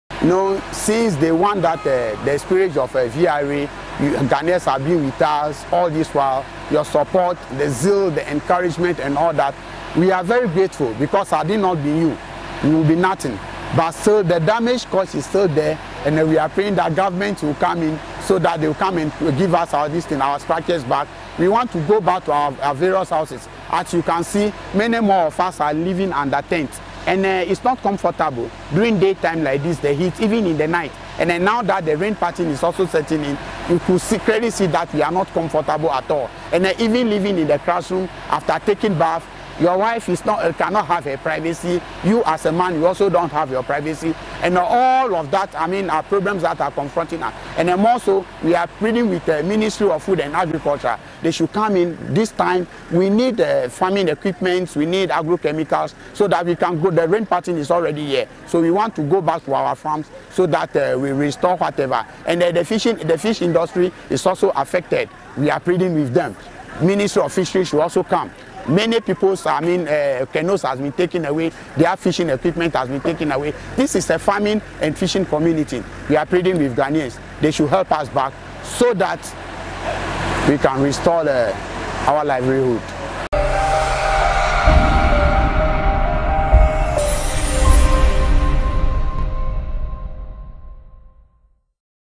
Assembly member for Mepe Electoral Area appeals for more support
News in Ghana